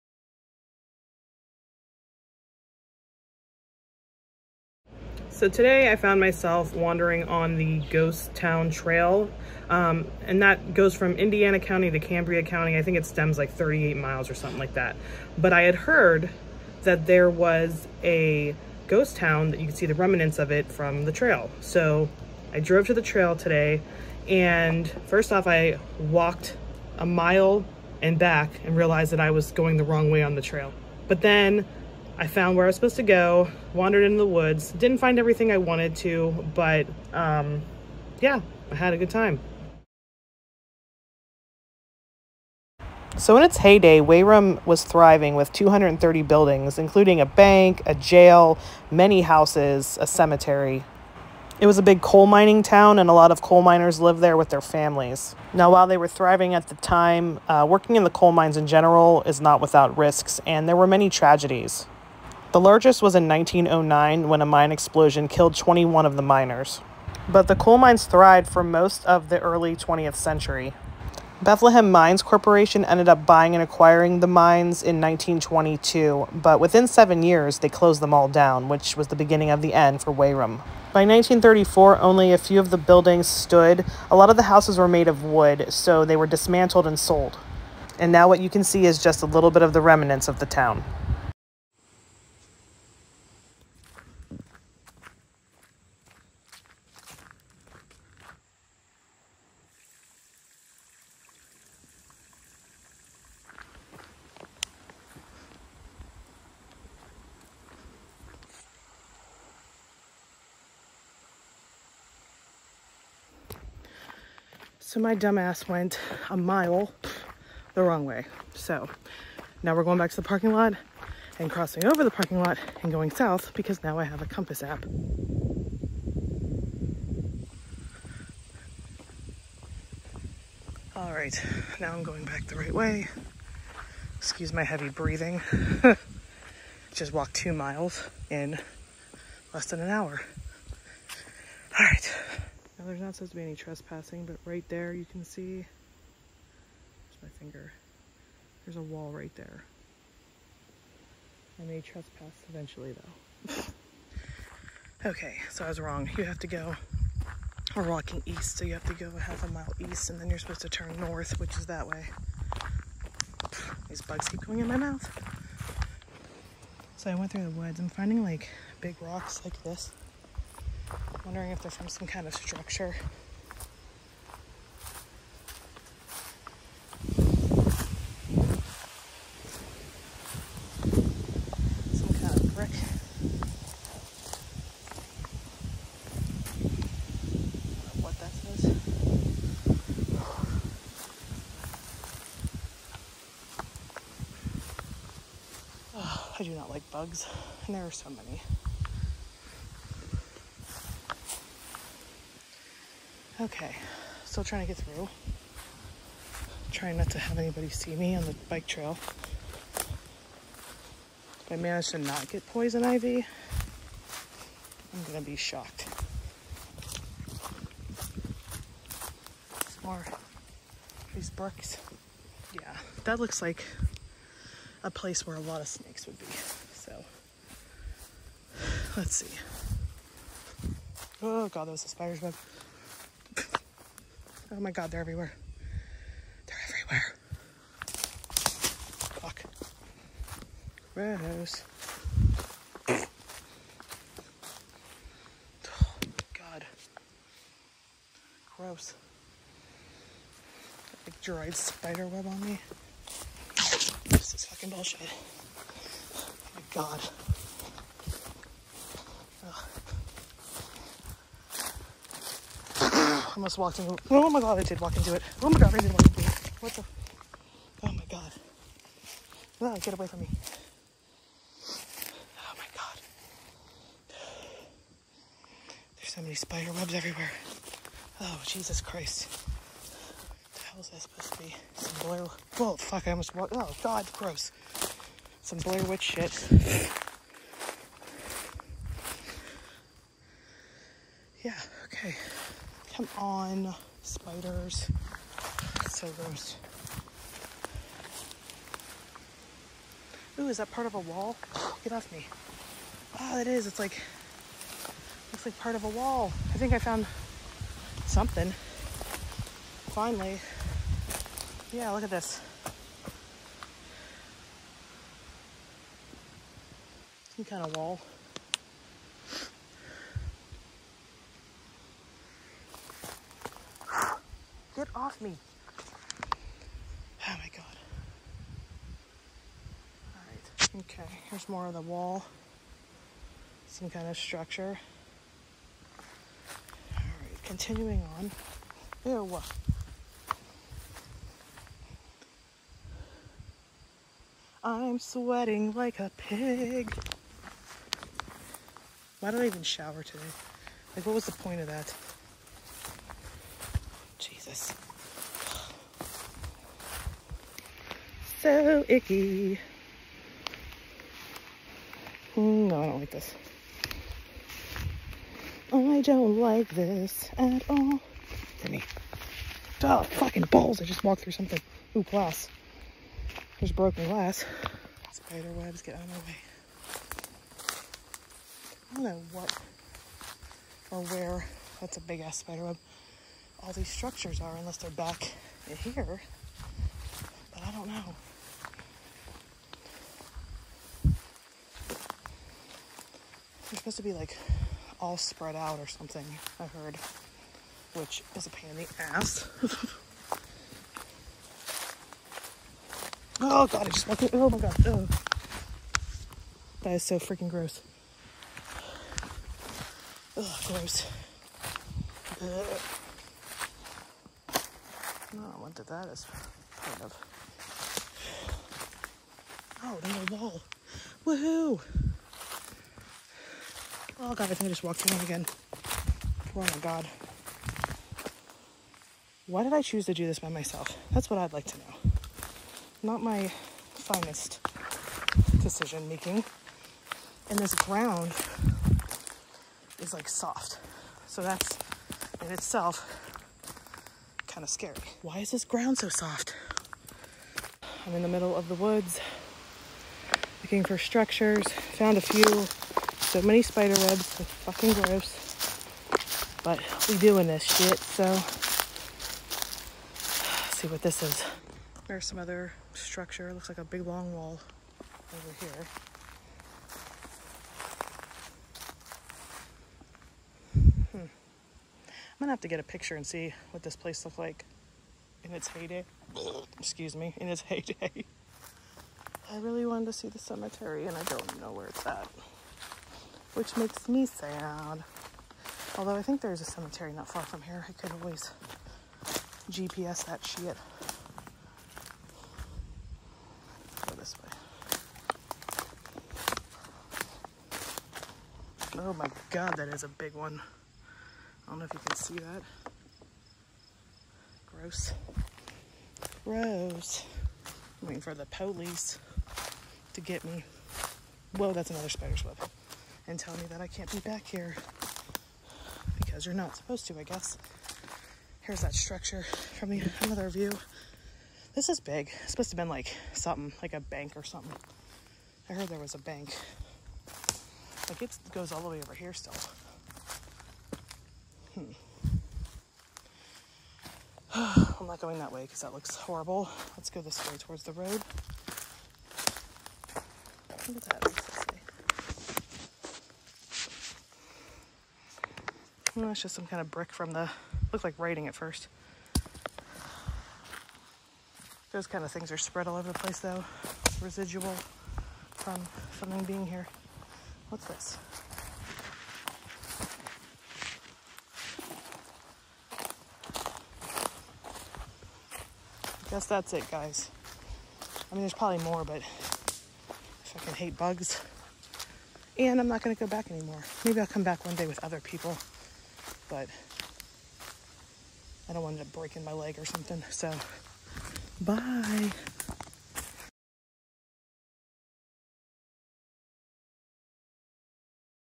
Wehrum Ghost Town in Appalachia
Jen goes on a search to find wehrum ghost town…